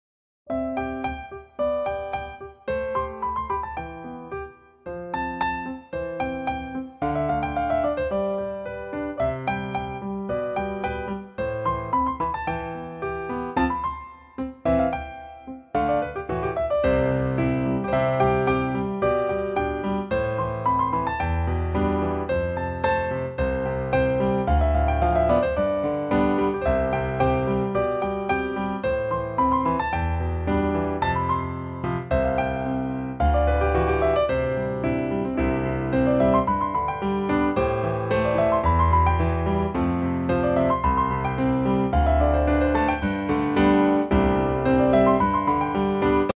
Training stick (three-headed stick)
training artifact of hand-eye coordination and reaction speed can effectively improve agility.training stick#speed#training artifact🔥